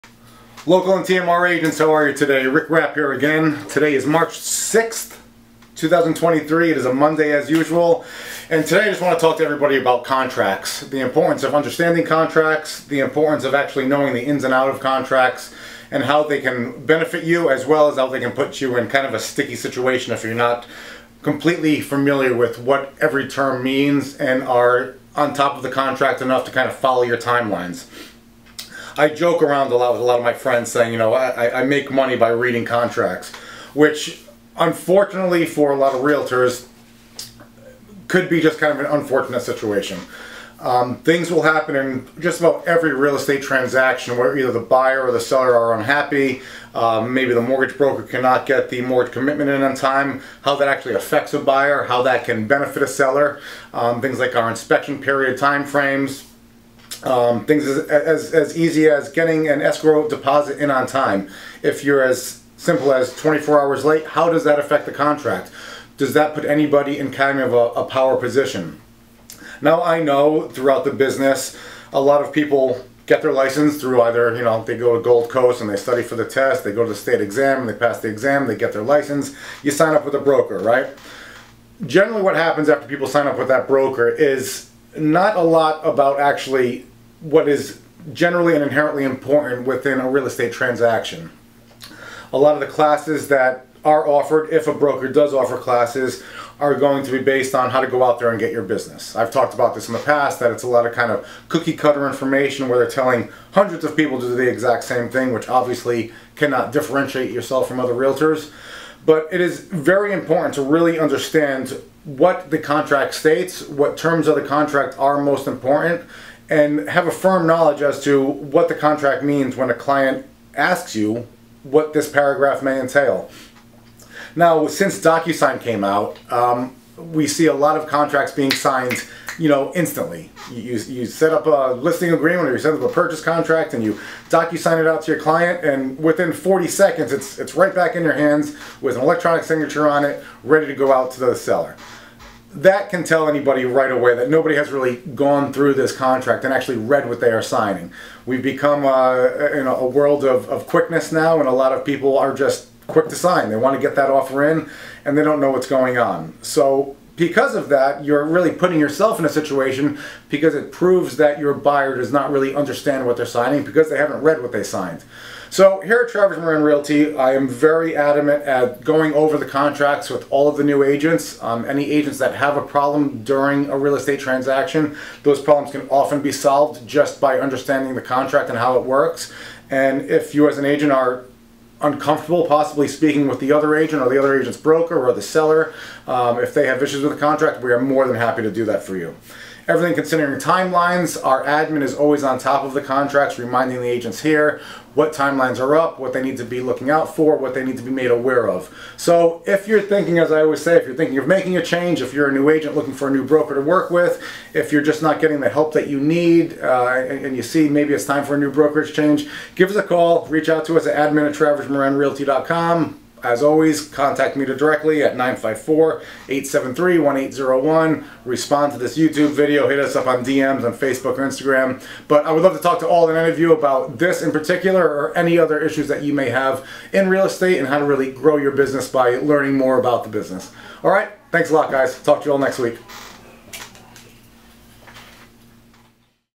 Rick Rapp discusses - The importance of understanding contracts
Many agents new or seasoned don't take the time to fully understand or explain the ins and outs of contracts to their clients, when issues arise they can be left in a sticky situation - here at TMR we are always here to help you understand the contract as well as how to use it to your advantage.